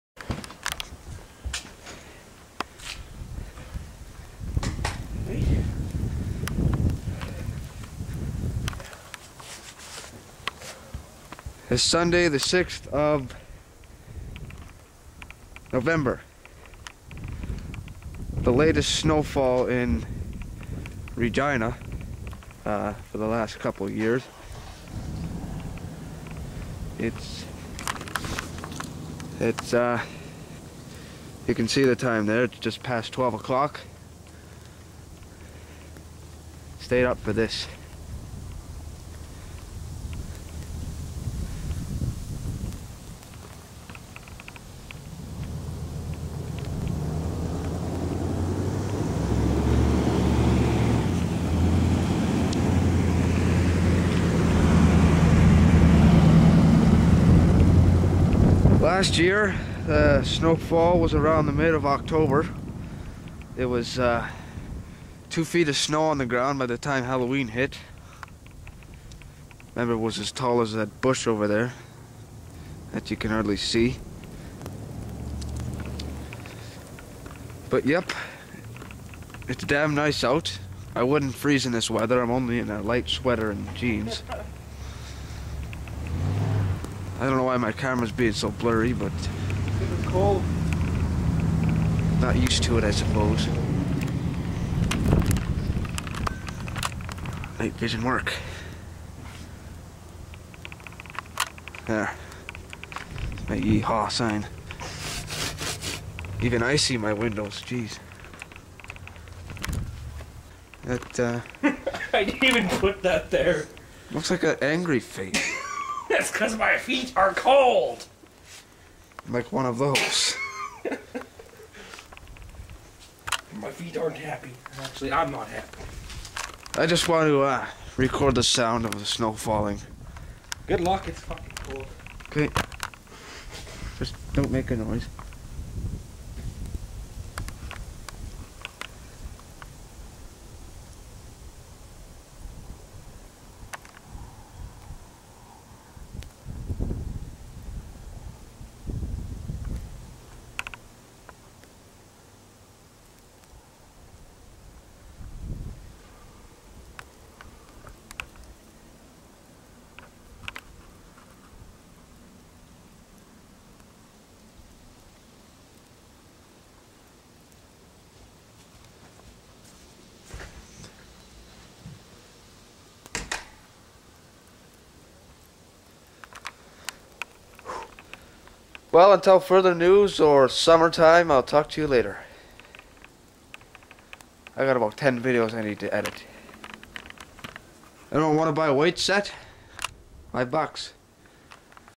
November 6 - First Snow Fall 2011 - HD
This is a really late upload as the snow is melting now but here is a video of the first snowfall, happend November 6th ar 11:53pm give or take.. Came way late this year.